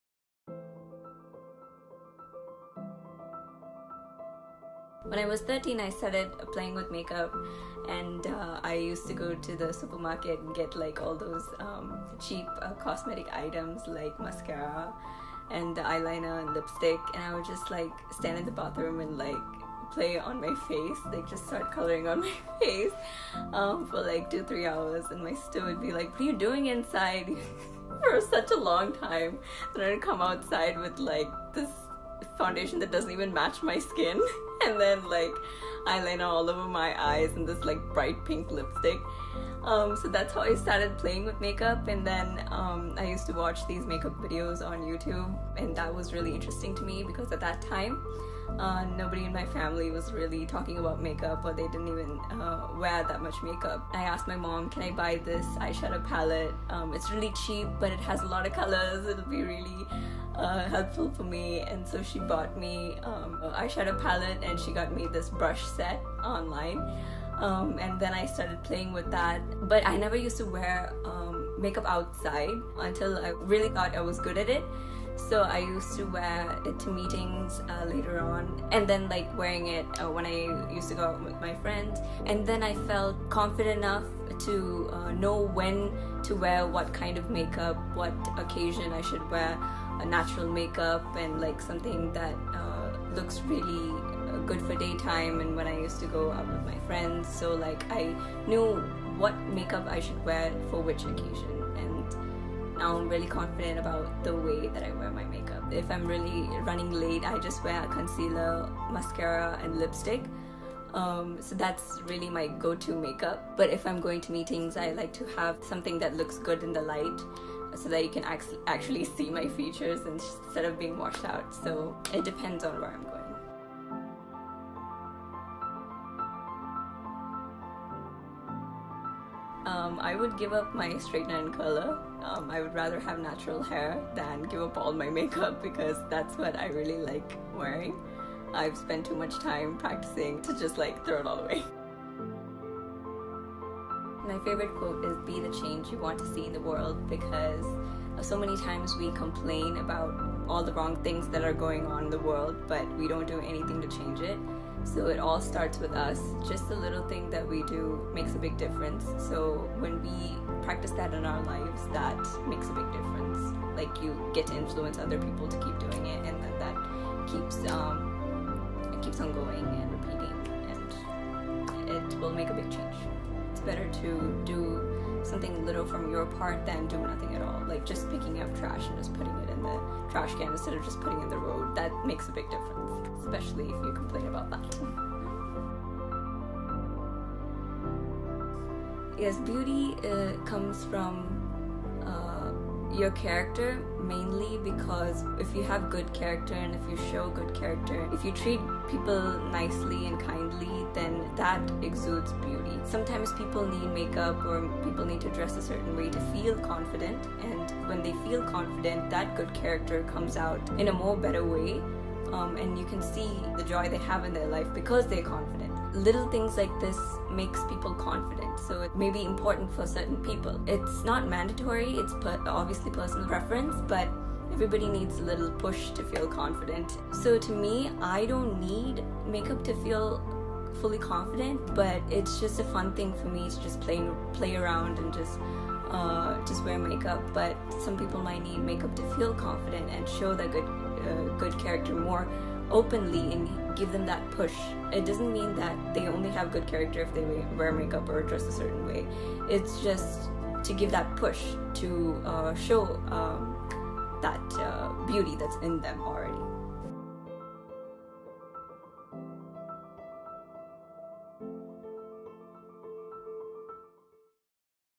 Q&A - Beauty, Makeup & Confidence | Stella Ramola | Just Being Real
In this video, I have answered questions that so many of you have asked on Instagram. I talk about m experience with beauty, makeup and confidence. Join the conversation and let me know how you are influenced by these topics, in the comment section below.

Listen to Stella Ramola’s music on all digital music platforms.